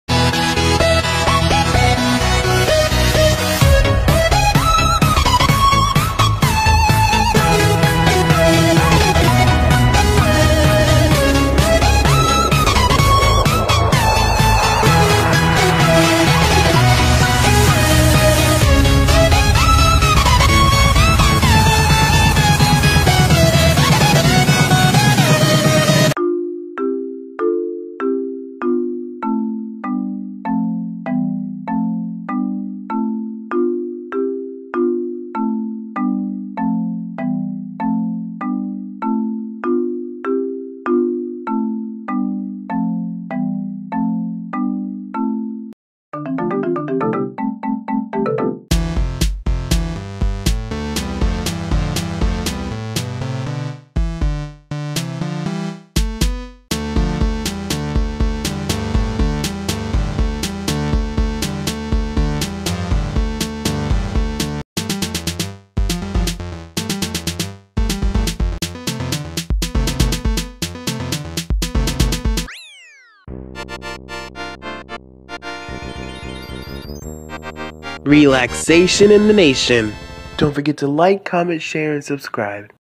Unnecessary Tracks I Made (Creationality Animations)
I do have some music that I made, it's just that it's not worthy of showing because it's more for anything you don't want to hear. This music was made in Chrome Music Lab. These were tests and I went ahead and try to implement it into Sonic 5 the game. I wanted to make it, but I decided to not do it and now this music is just left in my files without an actual spot to be in... until now. These are unnecessary songs that I had in my files I knew the song would come to be useful one day.